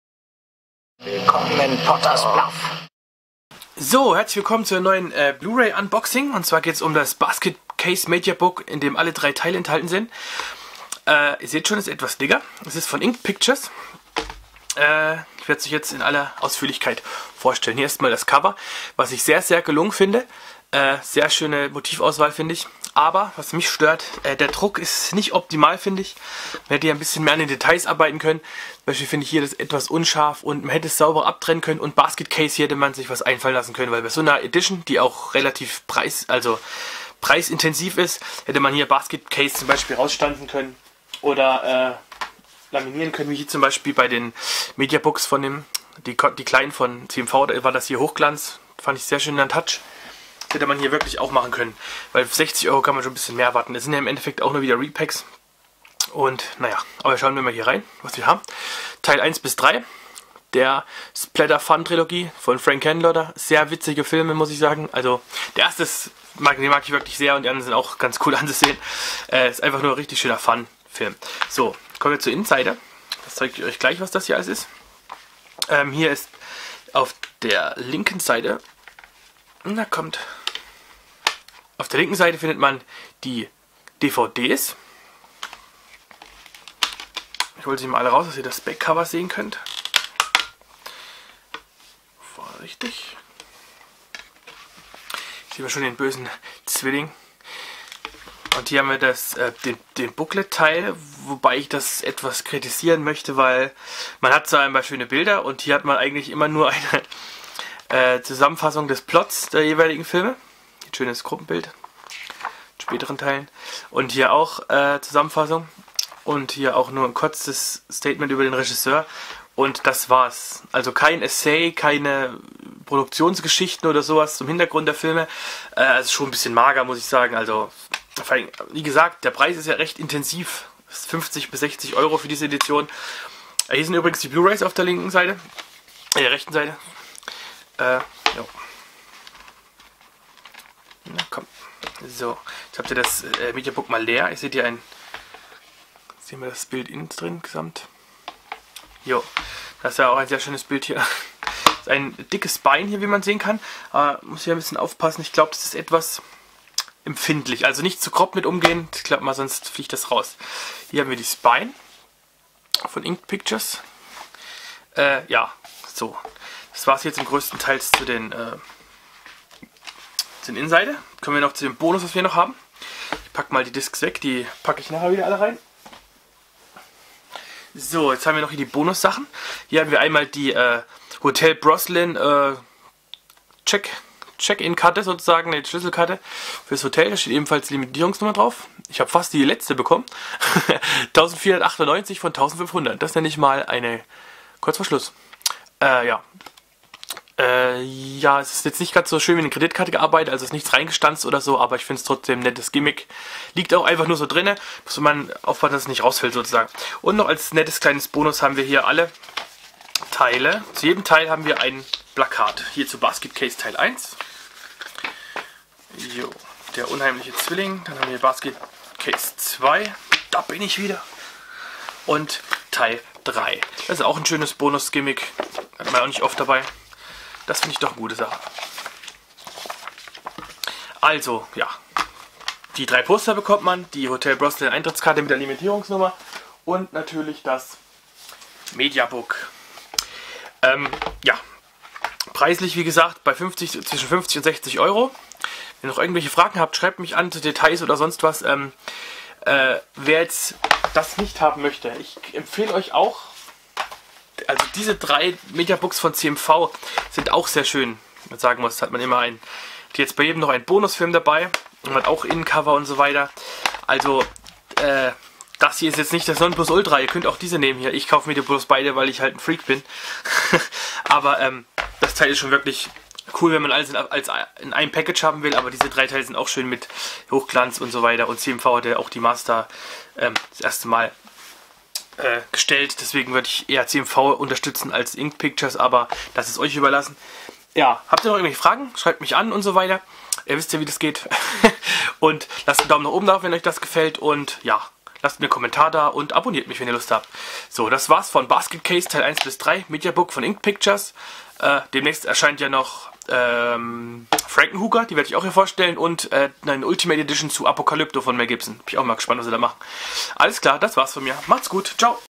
BASKET CASE Trilogie 6 Disc Mediabook Blu-Ray Inked Pictures Frank Henenlotter Unboxing
Meine Vorstellung des Basket Case Complete Trilogy Mediabooks
von Inked Pictures, inklusive 3 Minipostern Hotelkarte!
Limitiert auf 1.500 Stück auf jeweils 6 Discs verteilt!
Regie: Frank Henenlotter